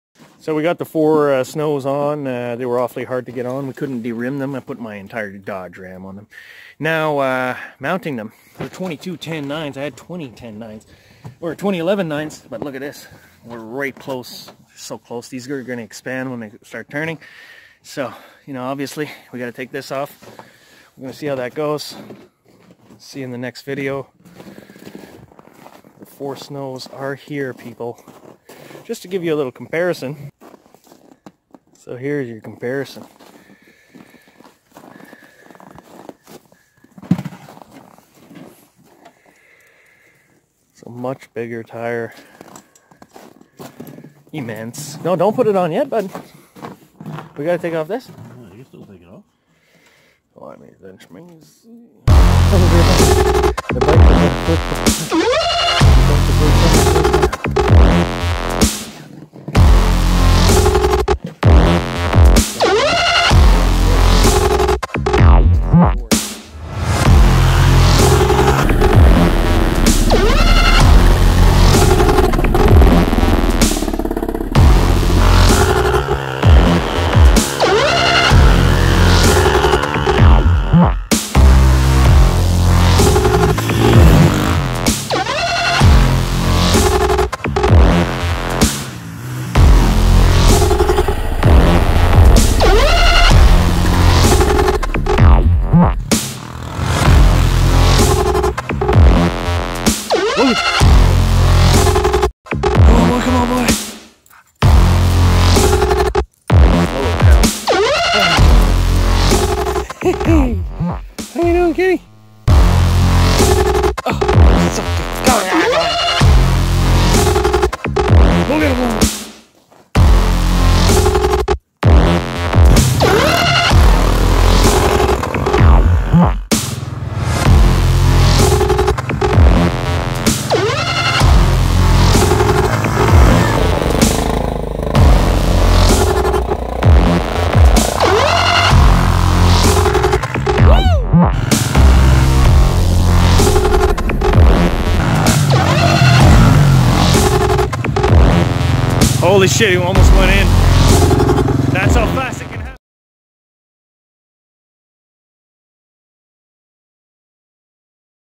Yamaha Raptor 350 on 4 snows!!
If ever you wanted to know if 4 snows were good lemme tell ya they freakin rock! FLEXHARVEY on 4 snows!!